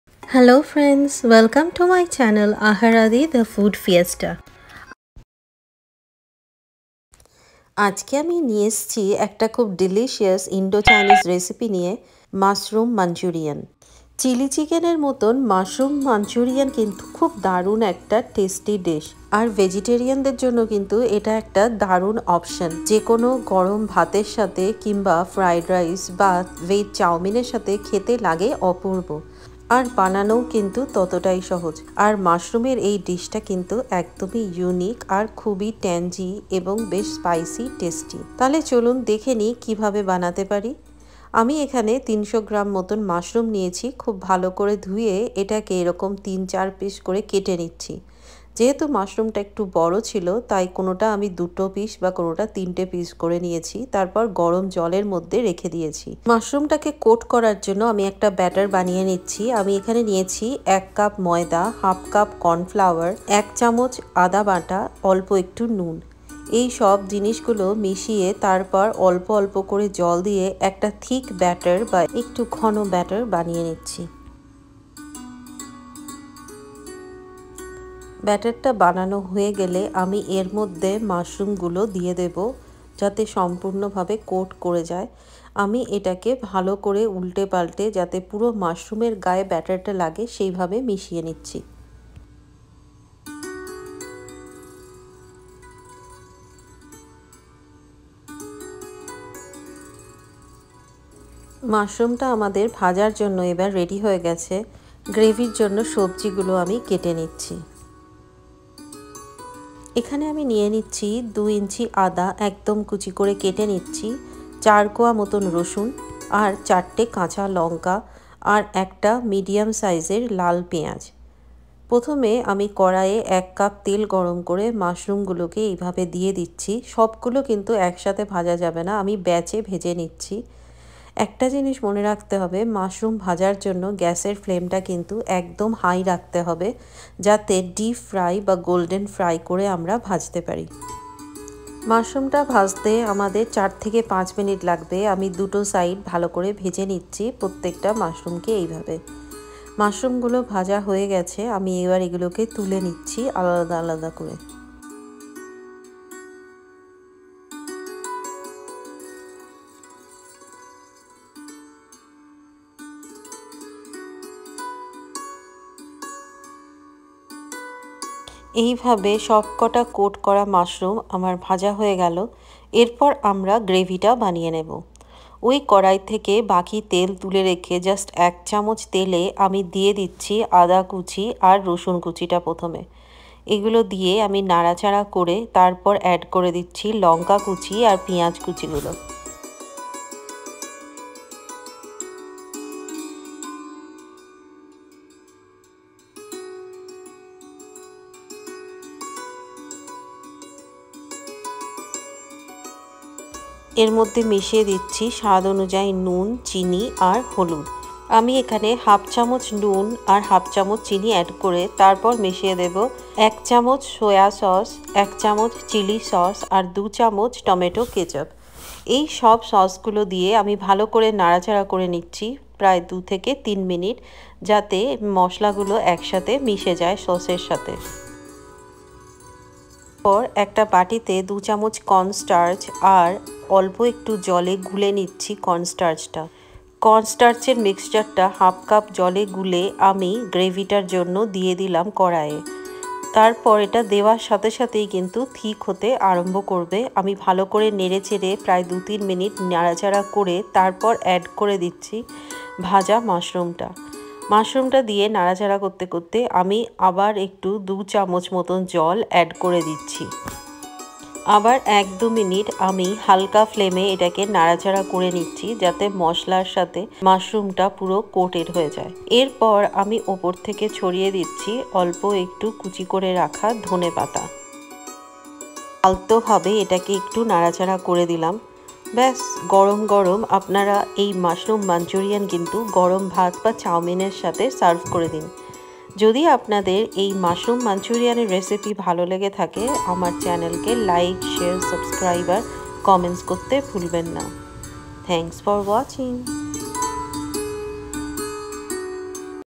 Mushroom Manchurian| Indo-Chinese recipe| মাশরুম মঞ্চুরিয়ান|How to Make Mushroom Manchurian Easily
Mushroom Manchurian| Indo-Chinese fusion recipe| মাশরুম মঞ্চুরিয়ান |How to make Mushroom Manchurian In Bengali
Manchurians are Indian-Chinese fusion recipes that are popular fast food items in India. Here I share my recipe for Mushroom Manchurian, which can be served as starters or side-dish with main-course.

Ingredients:

White Button Mushrooms - 1 box, (approx. 300 g);

Flour mixture- 1/2 cupof corn flour(corn starch) and all-purpose flour-1 cup (maida), 1 tsp salt, 1 tsp black pepper powder.(optionaol) ginger paste -1teaspoon

For the Sauce - 1 cup finely chopped onions, 2 tbsp finely chopped garlic; 1 tbsp finely chopped ginger; 1 tbsp finely chopped green chillies,

Corn flour slurry - 2 tbsp corn flour, 2/3 tbsp tomato ketchup, 1 tbsp red chilli sauce; 21tbsp soya sauce, 1/2cup water;

For garnishing - coriander leaves/cilantro, green spring onions, sesame seeds.